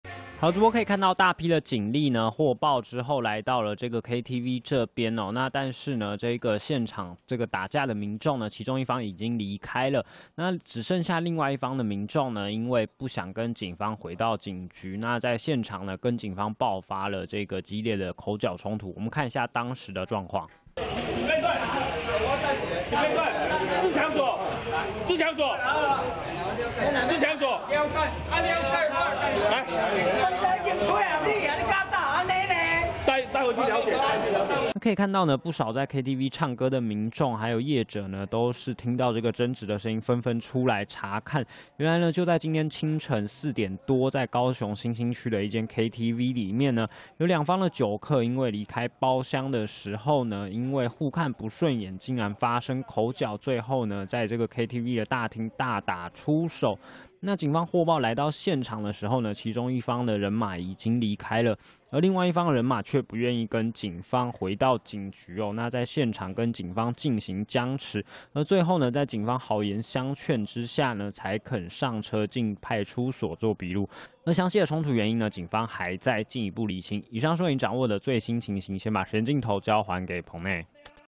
互看不順眼! KTV酒客互嗆大打出手│中視新聞20180730
高雄一間KTV凌晨發生鬥毆衝突，兩方酒客疑似喝醉酒、互看不順眼，在包廂外大打出手，最新消息 連線記者 許致誠